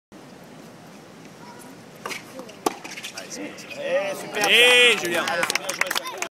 Benneteau Cheats to His Right, Sees His Anticipation Pay Off
Julien Benneteau upset 28th-seeded Radek Stepanek 6-4, 6-2, 4-6, 6-4 [R1, Court 13]